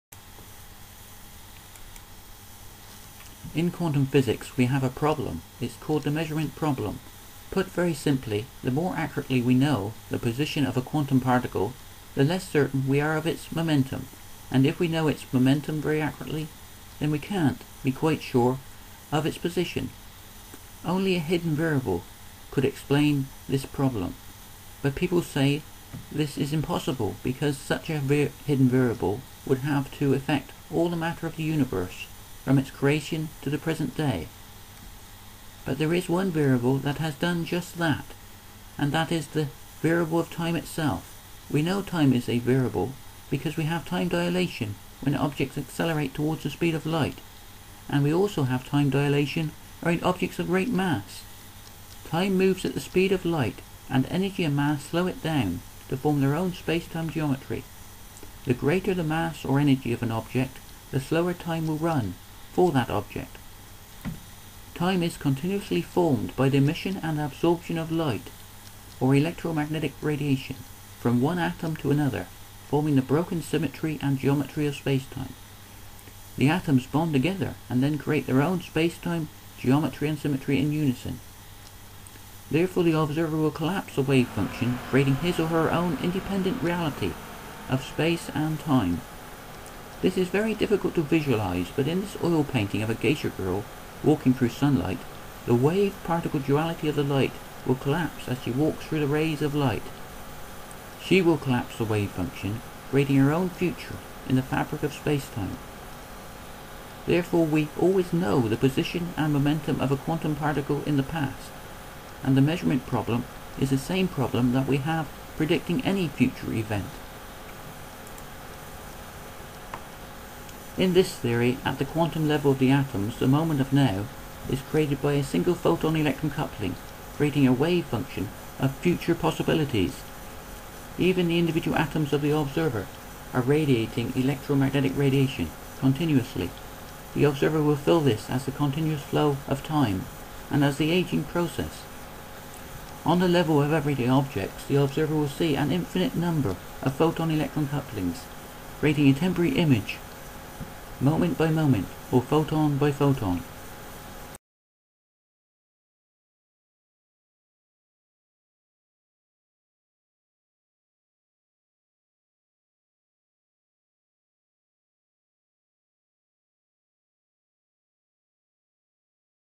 Is the measurement problem the same as any future probability?
Could the measurement problem of quantum physics be the same probability that we have with any future event? This is the idea puts forward by this video the idea that time itself is the hidden variable.   In this theory each new moment in time is formed by a single photon electron coupling creating a wave-function of future possibilities. We have a continuous process formed by the motion light forming the time continuum photon by photon or moment by moment.

Therefore Schrödinger's wave equation represents the forward passage of time itself and Heisenberg's Uncertainty Principle is the same uncertainty that we have with any future event.

The wave-particle duality of light can be explained as a process forming the passage of time. The continuous absorption and emission of light or EMR forms the arrow of time within a dynamically evolving universe.